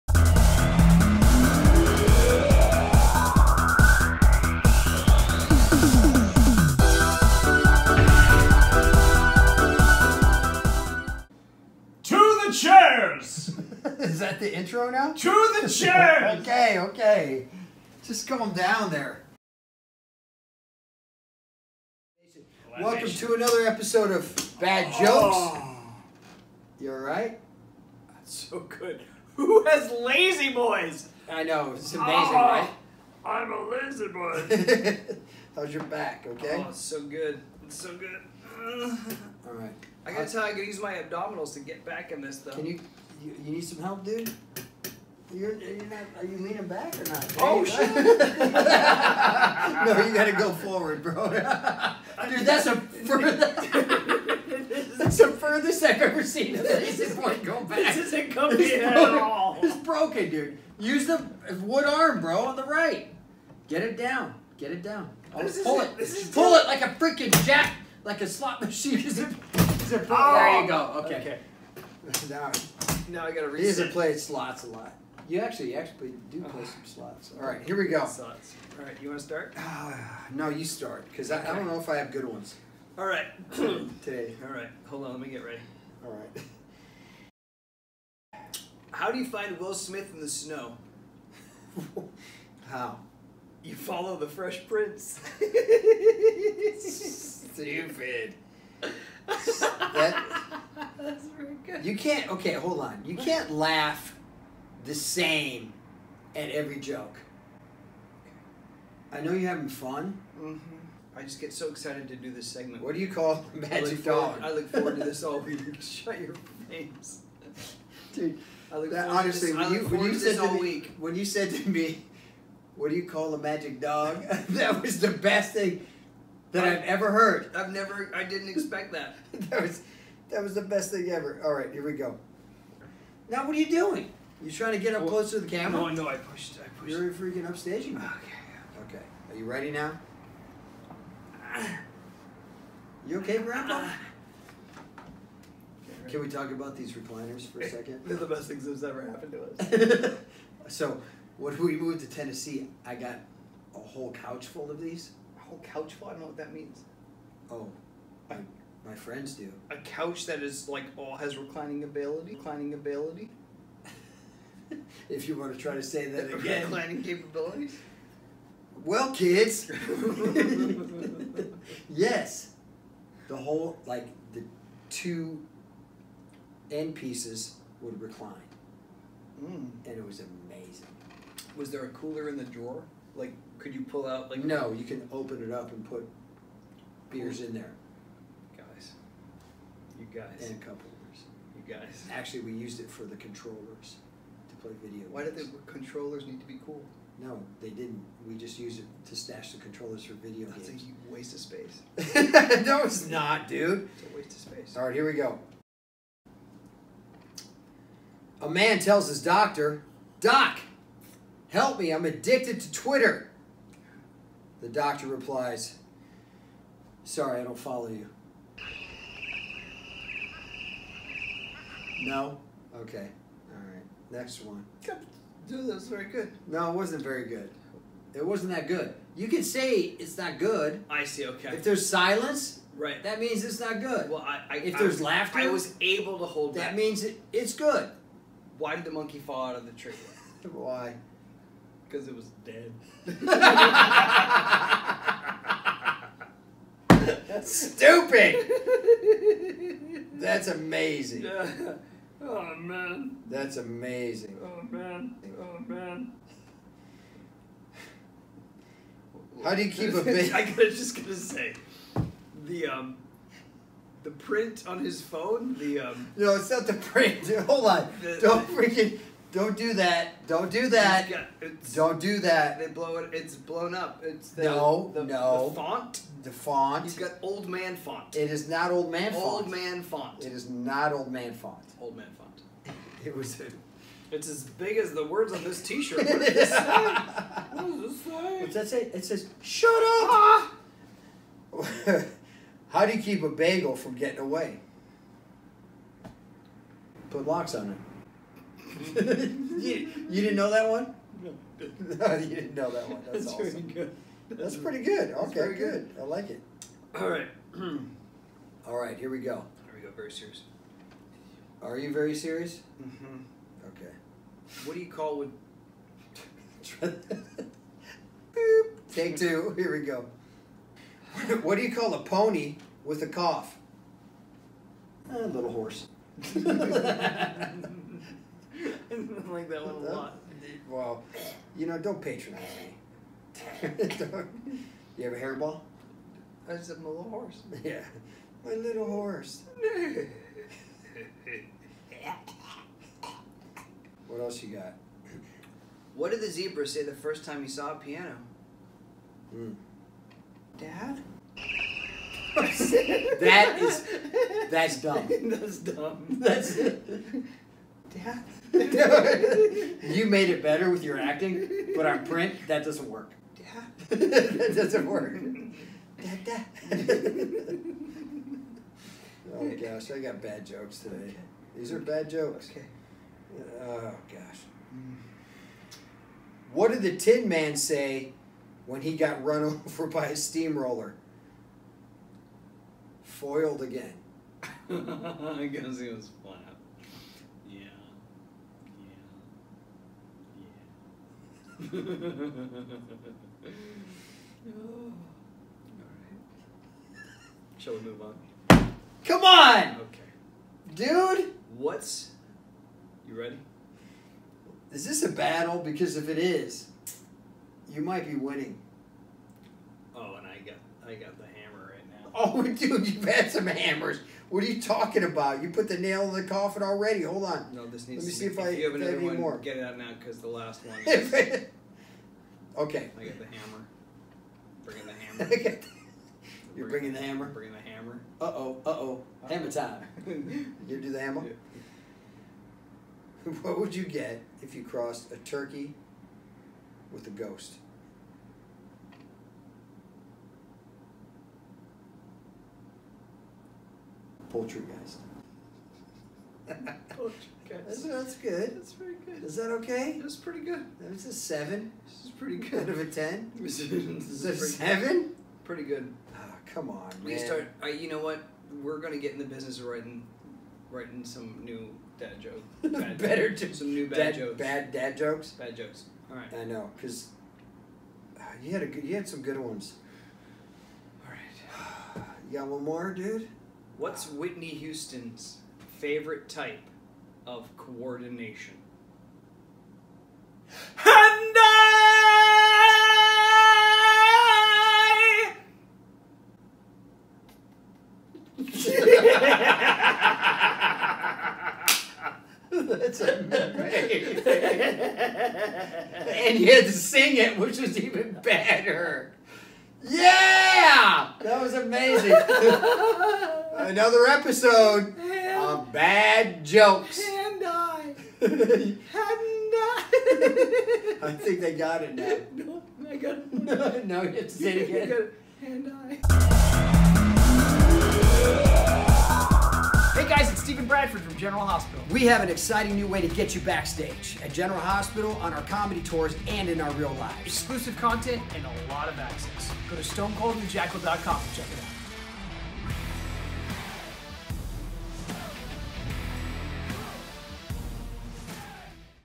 BAD (DAD) JOKES EP #4 with Steve Burton and Bradford Anderson
Knock Knock? Who's There? 2 Dummies! General Hospital's Steve Burton and Bradford  Anderson can't help but laugh telling each other BAD DAD Jokes! You will laugh!

Podcast, Tour Dates and Everything Else!